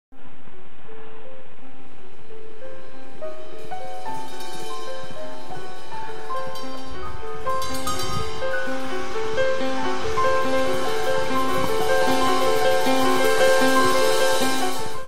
gallop
me riding my aunt's horse. Galloping the young stallion. This is the first time he's been let out. We're in Colorado, among the Sangre De Cristo Mountain range.